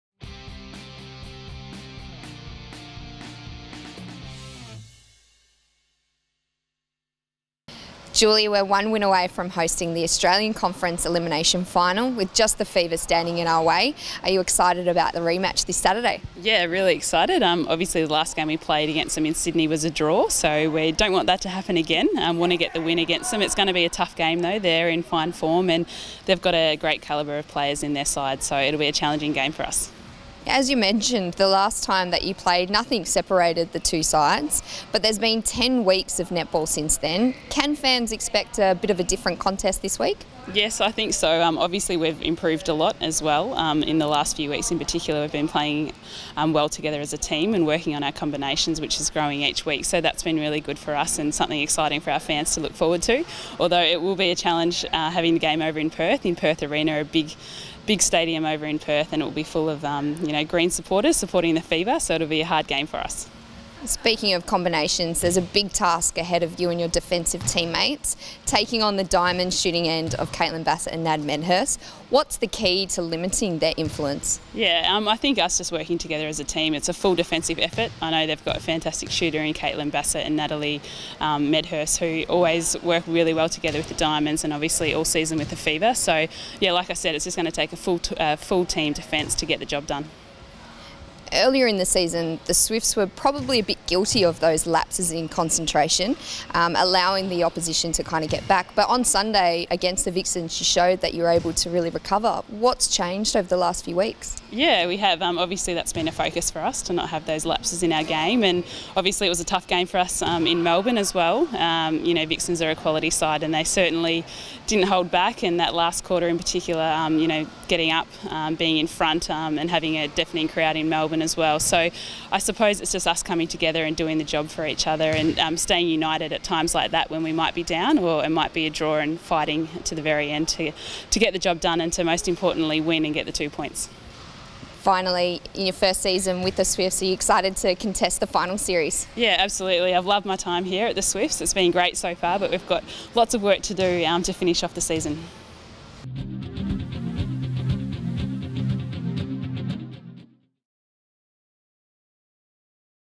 NSWSwiftsTV: Corletto admits tough task ahead
Superstar defender Julie Corletto is excited for the NSW Swifts v West Coast Fever rematch this Saturday 30 May, when she and her teammates travel to Perth Arena aiming to win the right to host the Australian Conference Elimination Final.

NSWSwiftsTV caught up with the Australian Diamonds defender at Swifts training during the week, who said the NSW outfit were keen to rectify the Round 4 draw that resulted the last time the Swifts met the Fever.